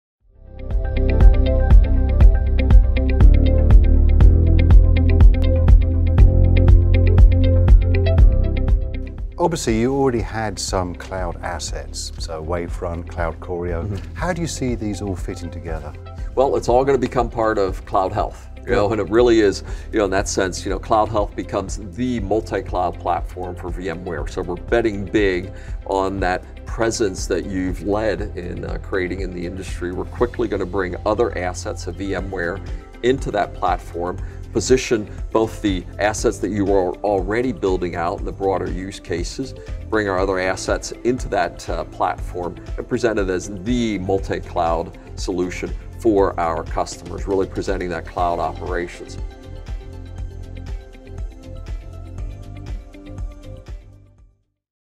How Will CloudHealth and VMware Cloud Fit Together?
VMware CEO Pat Gelsinger says that VMware will incorporate multiple cloud services
into CloudHealth to create the ultimate multi-cloud solution for customers.
Wavefront by VMware, VMware Secure State and VMware Cloud Automation Services will
join forces with CloudHealth Technologies, an established and industry leading solution for
managing native public clouds, to deliver the most complete and robust set of capabilities
for managing multi-cloud environments that include hybrid and native public clouds.